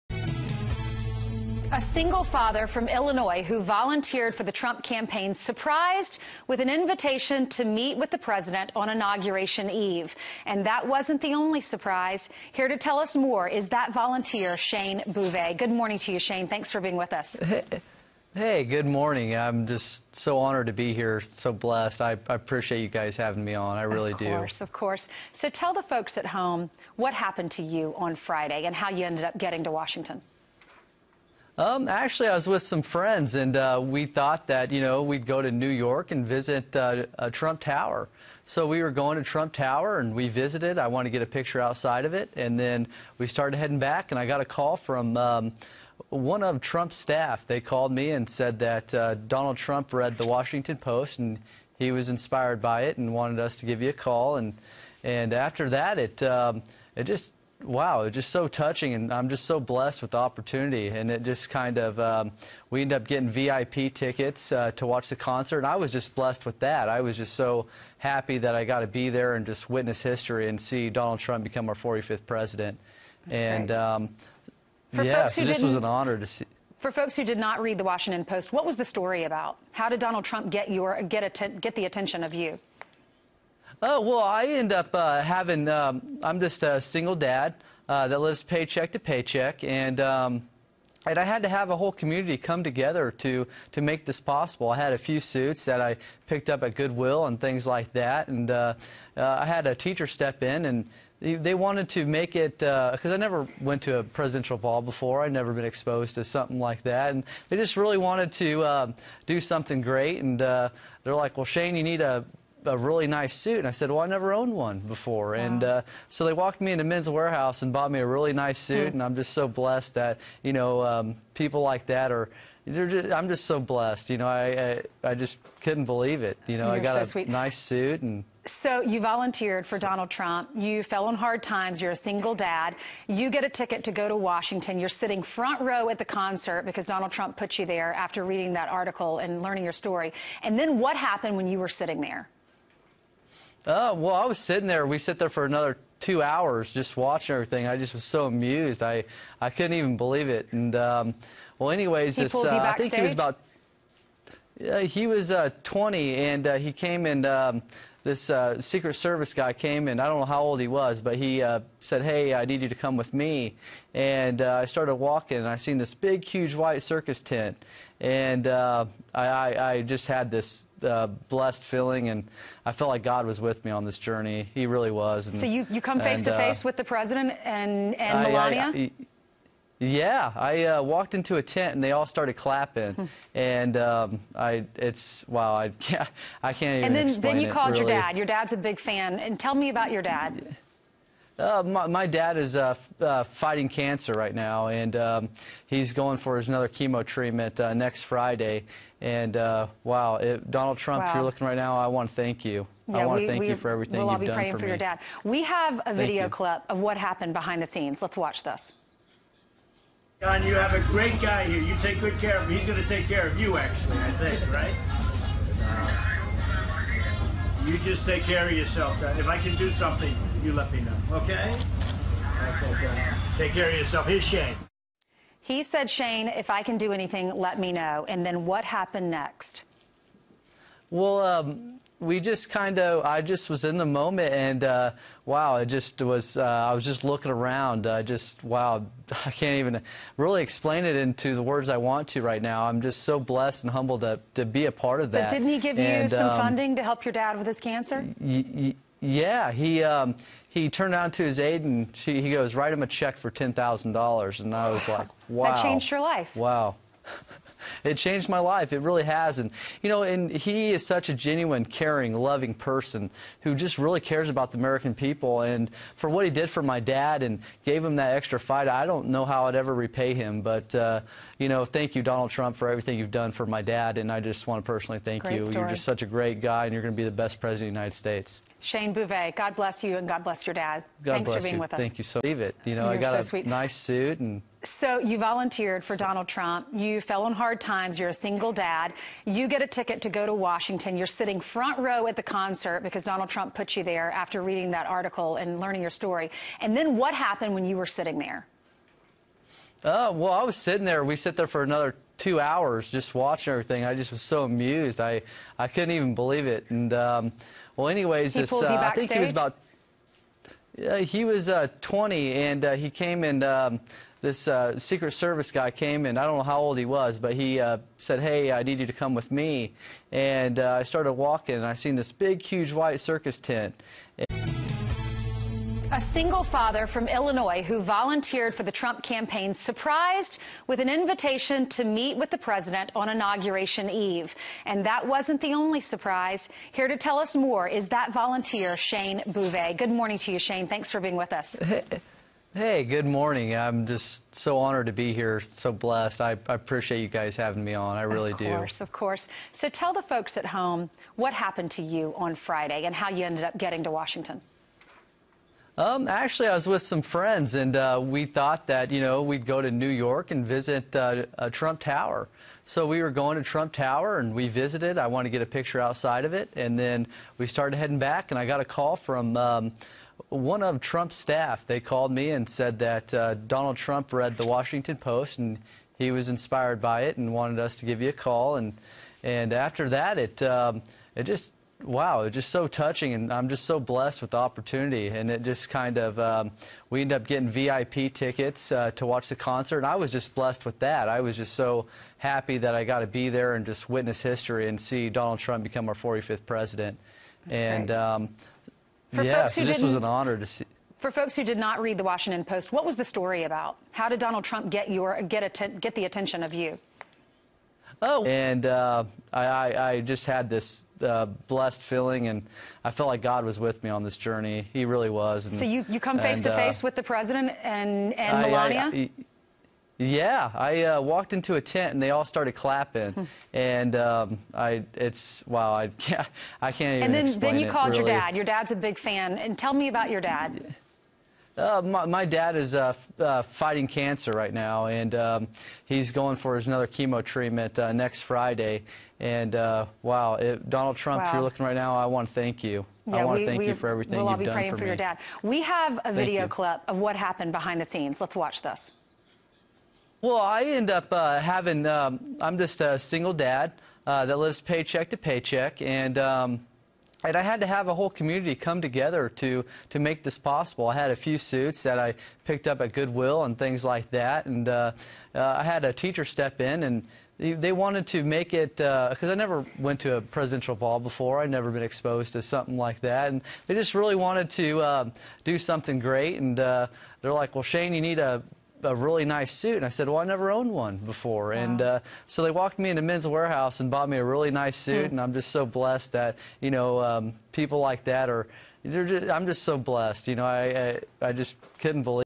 Trump invites a struggling dad to inauguration, offers $10K
LIVE NOW !! DONALD TRUMP INAUGURATION LIVE, DONALD TRUMP INAUGURATION, DONALD TRUMP BALL .